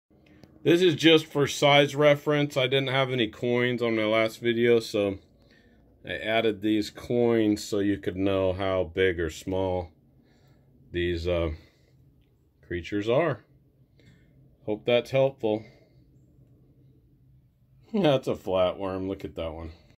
Human Flat Worm Parasites? Graphic!
Austin Regional Clinic Lab in Cedar Park performs stool test w results of no parasite. ??